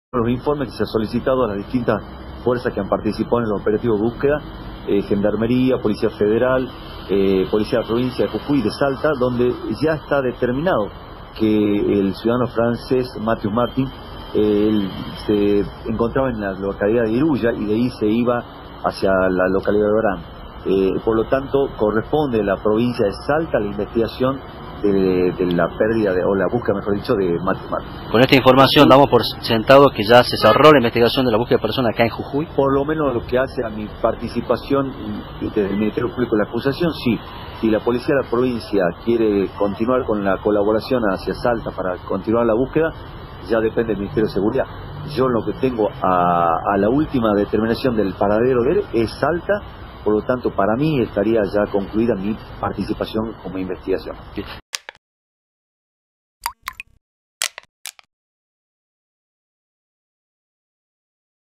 La búsqueda del mochilero francés quedó a cargo de la Justicia salteña
Las pruebas indican que Mathieu Martin se encontraría en territorio salteño, por lo que la fiscalía jujeña dio por cerrada su participación en la investigación.